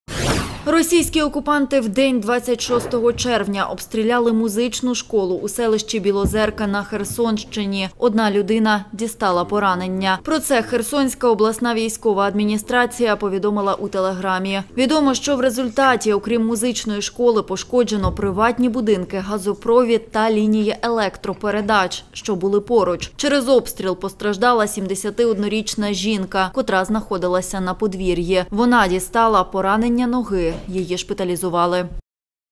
🤬🤬росіяни вдарили по Херсонщині: ПОРАНЕНО ЖІНКУ, пошкоджено будинки
Російські окупанти вдень 26 червня обстріляли музичну школу у селищі Білозерка на Херсощині, одна людина дістала поранення. Про це Херсонська ОВА повідомила у Telegram.